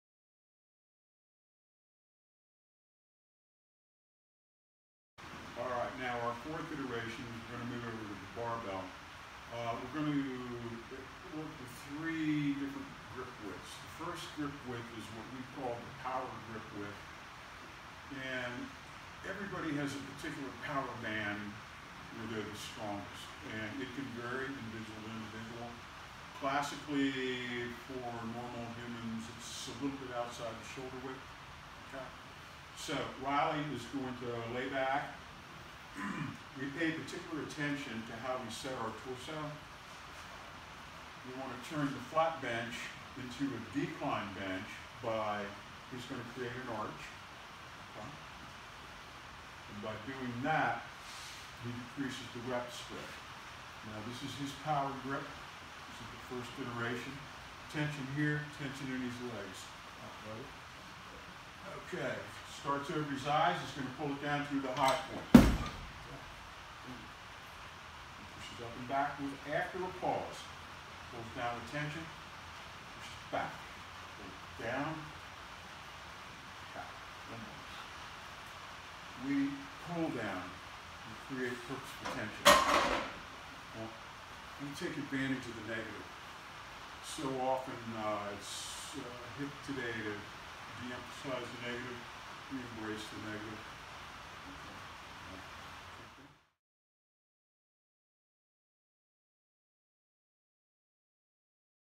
Barbell Flat Bench Press Technique - Power Grip | Marty Gallagher
- Barbell bench press using the power grip for maximum poundage. Karwoski Approved Power Bar used to make this video.